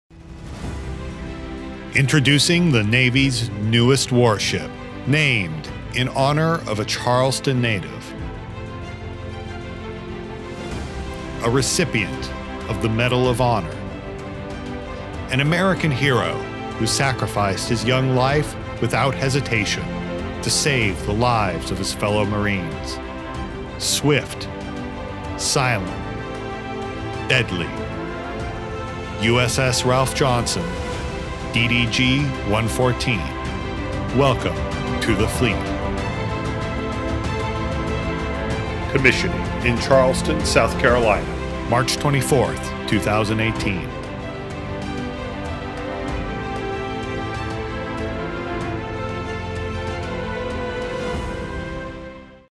USS Ralph Johnson (DDG 114)
The future USS Ralph Johnson (DDG 114) commissioning March 24, 2018 in Charleston, South Carolina.

U.S. Marine Corps Pfc. Ralph Johnson, a native of Charleston, posthumously received the Medal of Honor for his heroic actions during the Vietnam War. Johnson used his body to shield two fellow Marines from a grenade, absorbing the blast and dying instantly in March 1968.